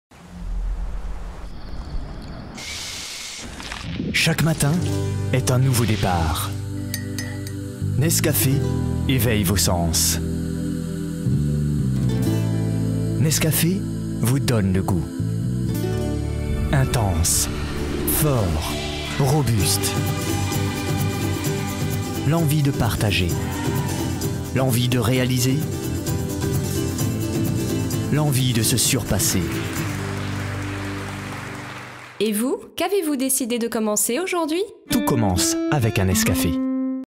NESCAFE Masterbrand Pacifique - 37' -2015
Nouveau film Masterbrand dédié au marché Polynésien - production d'une version pour les marchés : Nouvelle Calédonie - FIDJI - Nouvelle Guinée.